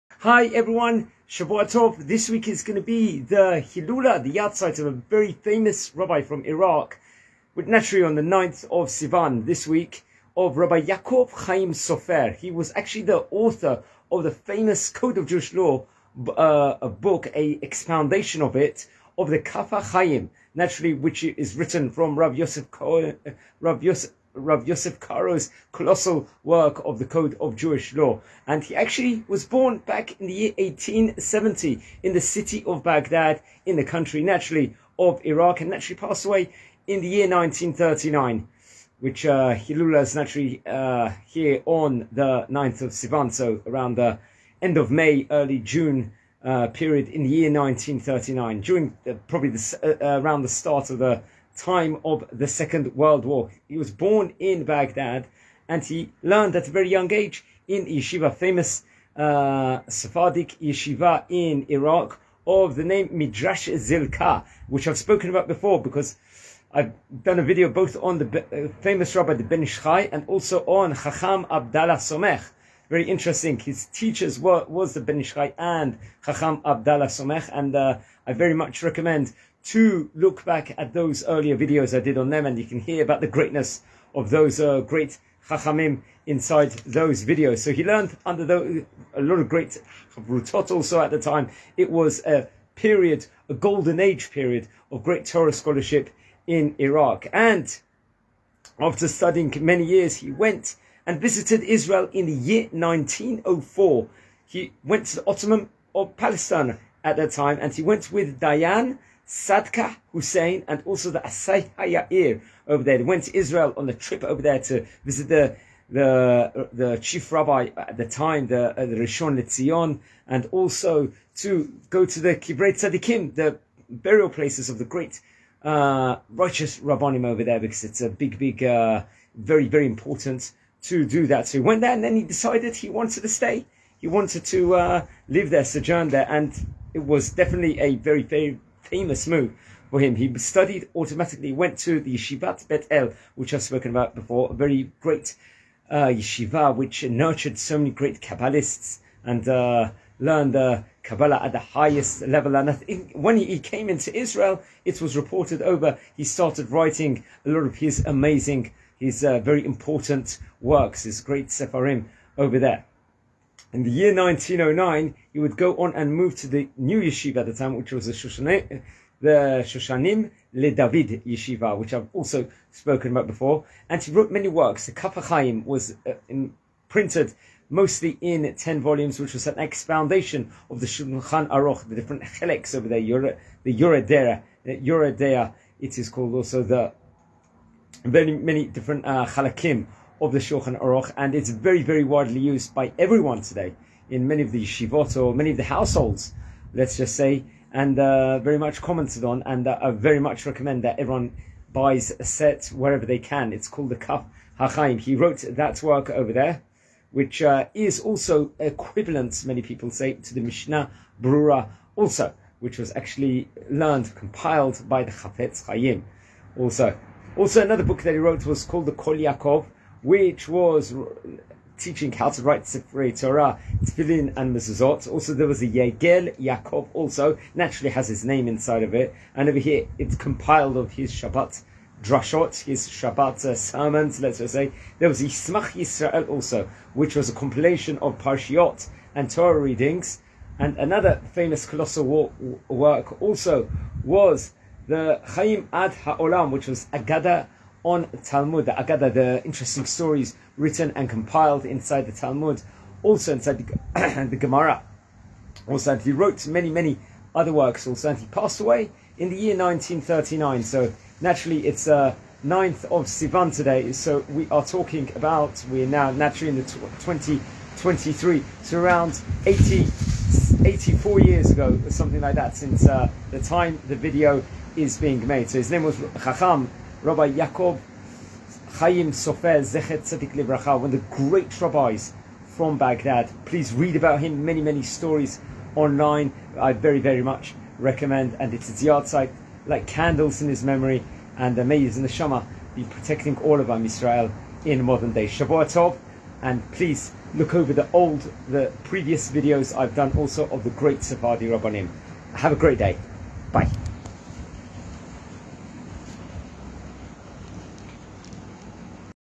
Rabbi Yaakov Chaim Sofer Z'tl - הרב יעקב חיים סופר - כף החיים - Kaf HaChaim - Great Rabbi from Iraq
Rabbi Yaakov Chaim Sofer Z'tl was born in Baghdad, in Ottoman Iraq. He studied the Torah under Chacham Abdallah Somekh and the Ben Ish Hai. He is the author of the Kaf Hachaim.